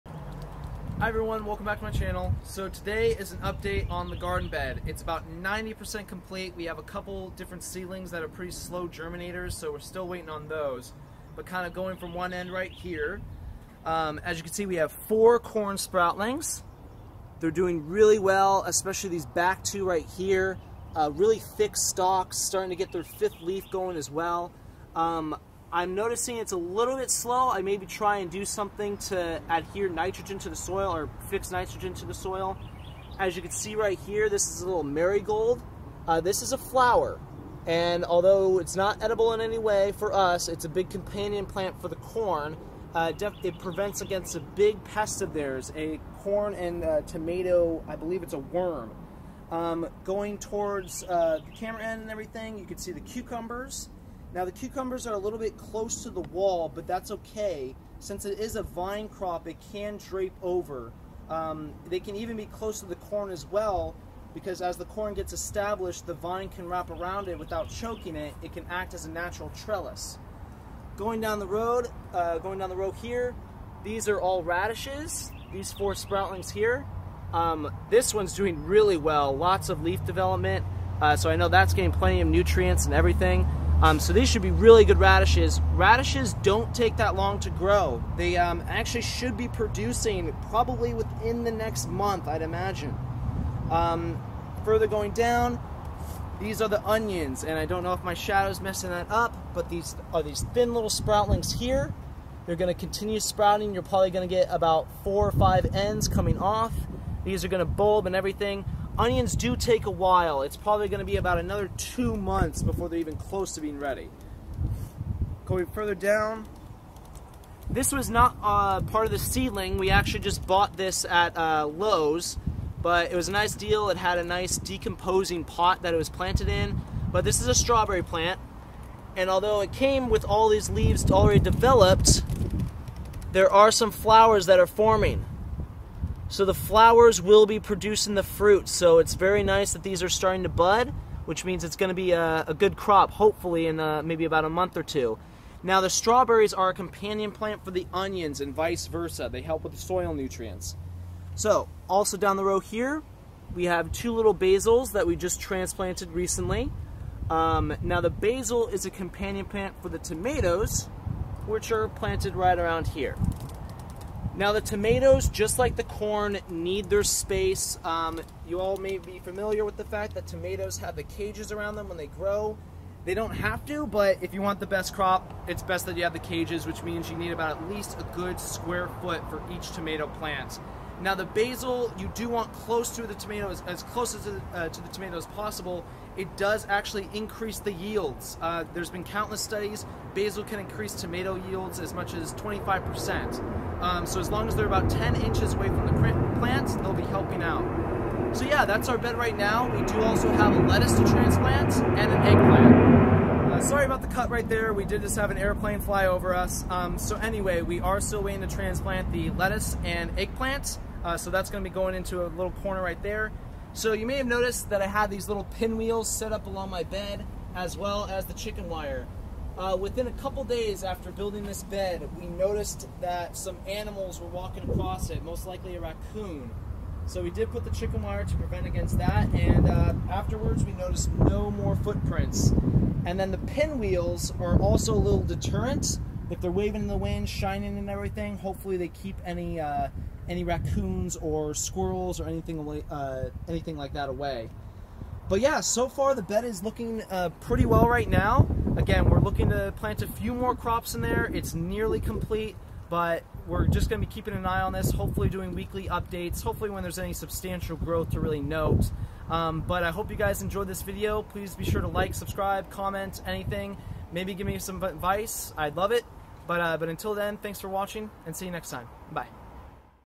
Garden Bed Update
This is about one week in since I began transplanting my cells. I started with corn, then soon added radish, tomato, marigolds and so on. They all had about a 2 to 3 day period before they started getting settled and began growing again. It has been fairly cold here recently. My next video will be an update when there is some substantial growth. First to be harvested will definitely be the radishes.